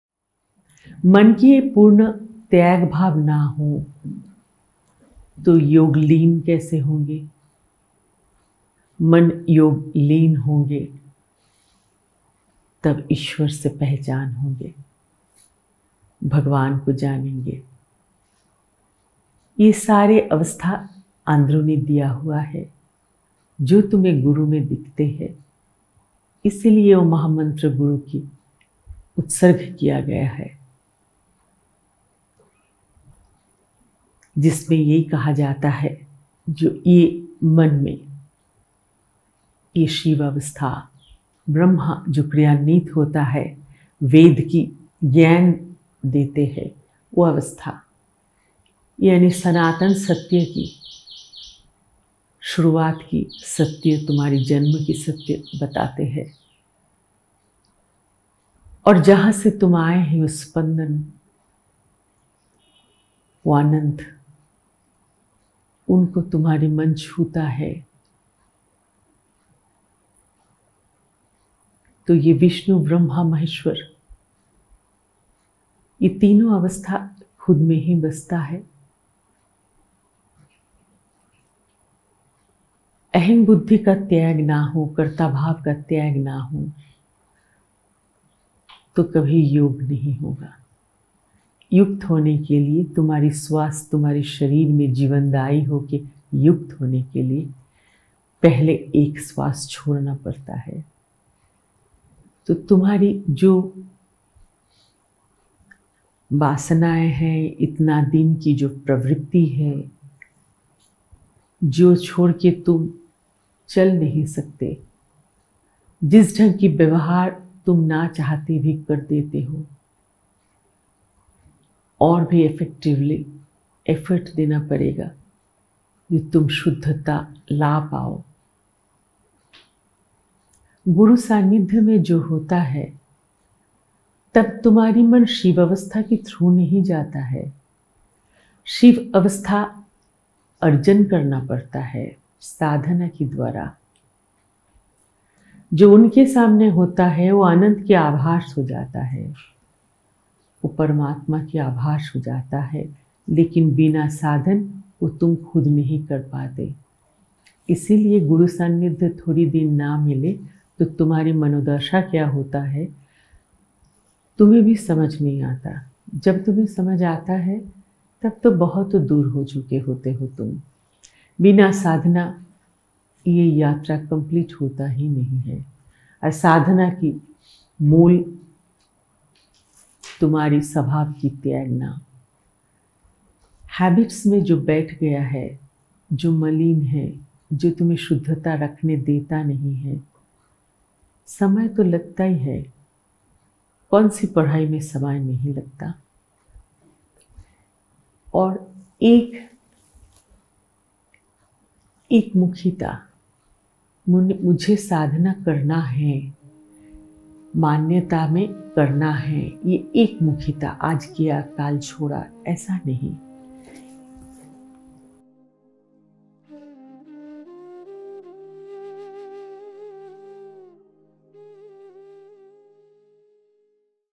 YOG Through GURU SANNIDHYA & SADHANA | Tyag Yog | SHREE GURU MAASHREE
Excerpt is from "Tyag Yog" audio CD spoken in Hindi.